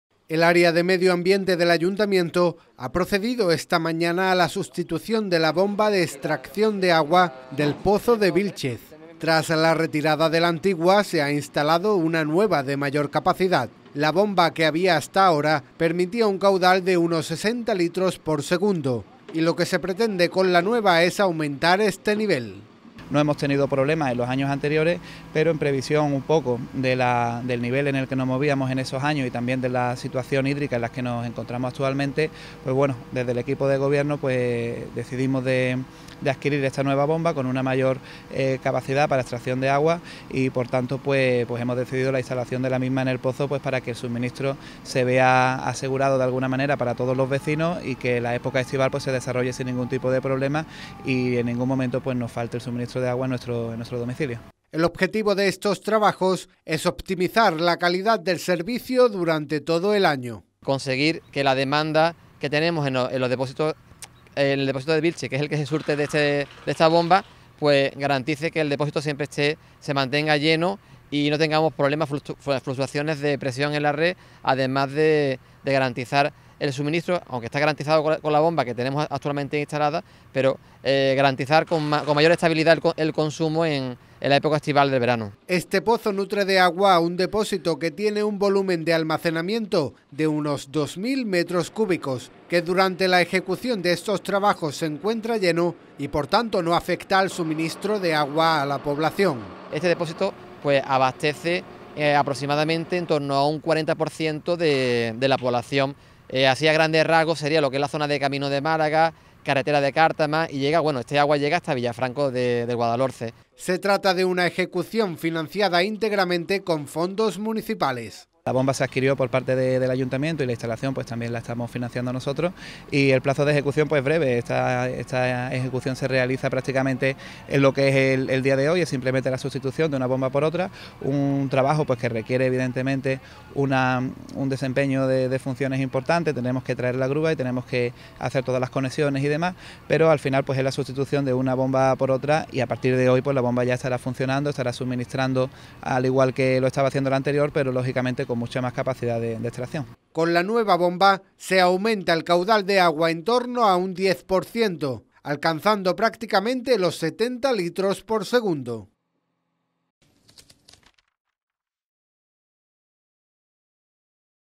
El Ayuntamiento sustituye la bomba de extracción de agua del pozo de Vílchez por una de mayor capaci
El área de Medio Ambiente del Ayuntamiento ha procedido esta mañana a la sustitución de la bomba de extracción de agua del pozo de Vílchez. La nueva bomba permitirá un 10% más de caudal que la anterior.